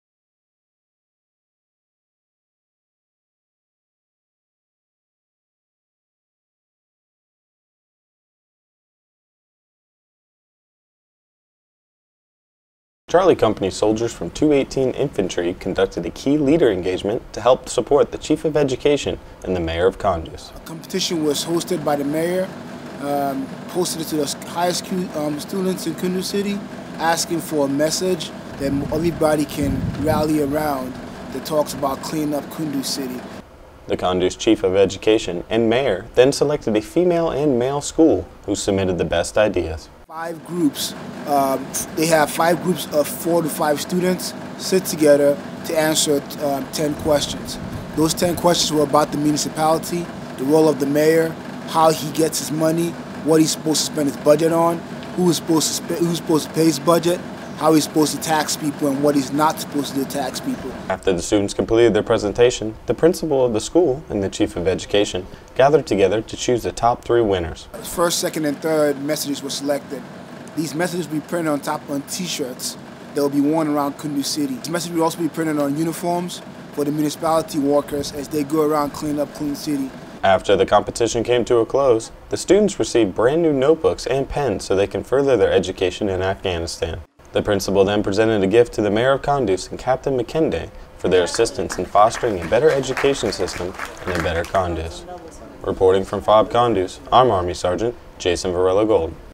We Are Kunduz
Knowledge is the best gift anyone can give a student in a school and in this story Army Sergeant Jason Varela-Gold explains how the key leaders in Kunduz want to help spread the wealth as well. Sound bite includes: Army CPT Omololu Makinde, Commander Charlie Co. 2-18 IN